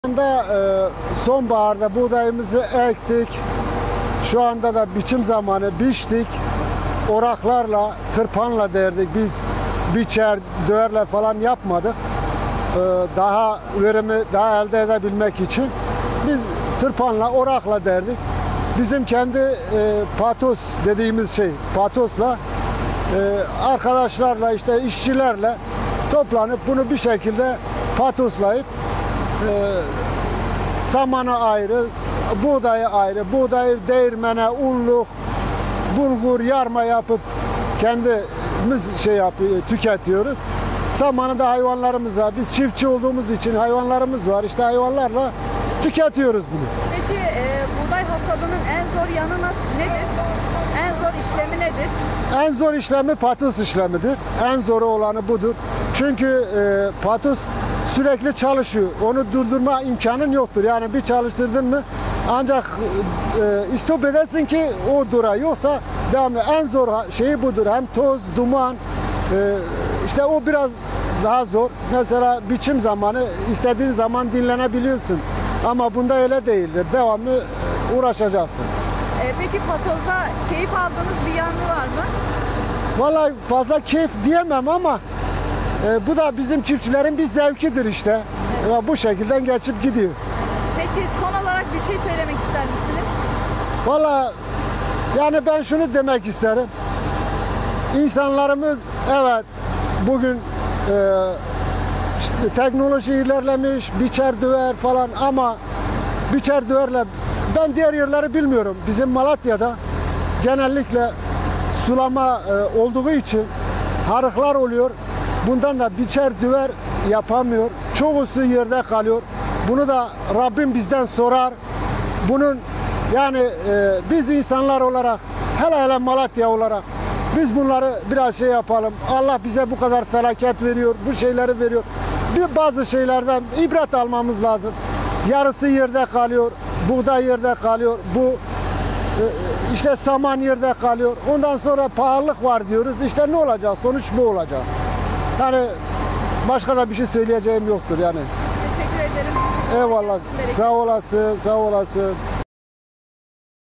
Malatya’da buğdayda düşük verim
Malatya’nın Battalgazi ilçesinin Hatunsuyu Mahallesi’nde buğday hasadı başladı.